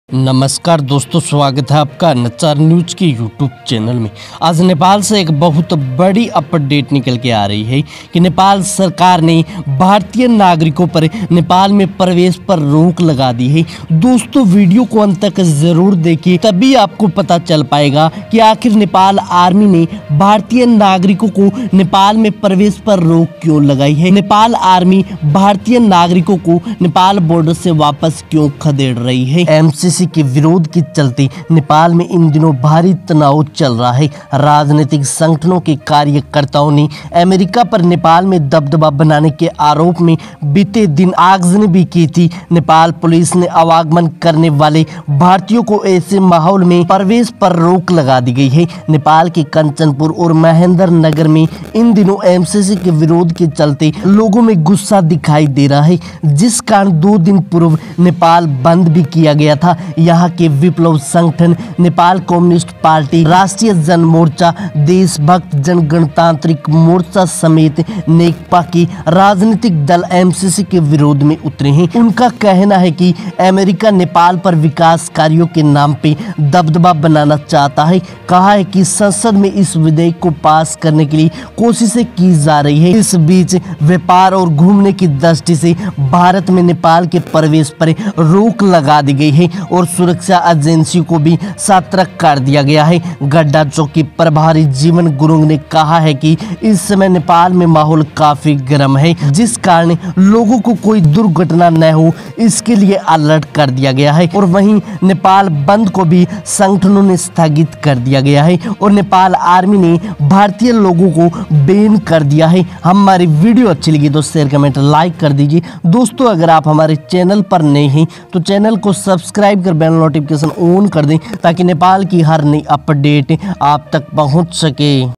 नेपाल आर्मी ने भारतीयों के प्रवेश पर लगाई रोक!Nepali Army Took Big Step on india Due to MCC!Napal
नेपाल आर्मी ने भारतीयों के प्रवेश पर लगाई रोक!Nepali Army Took Big Step on india Due to MCC!Napal Army Proud!🇳🇵🇳🇵🇳🇵